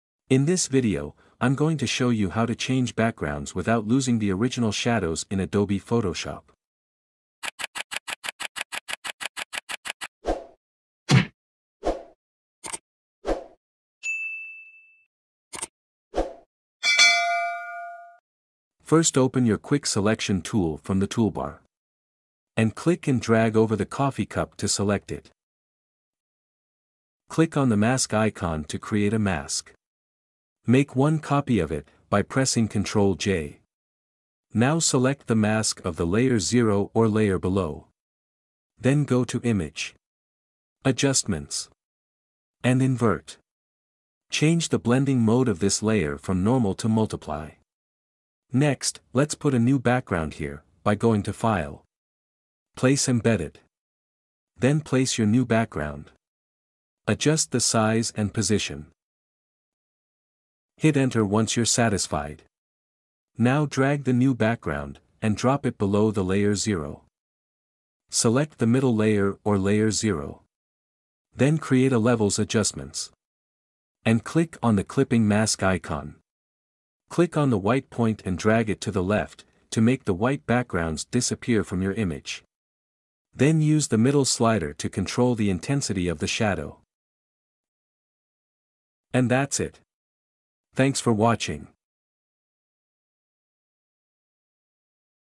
The Right Way To Change/Delete Backgrounds without Losing the Shadows - Photoshop Tutorial
In this step-by-step Photoshop tutorial, you'll learn to change backgrounds without losing the original shadows in photoshop. I hope this lesson helps.🙏🤗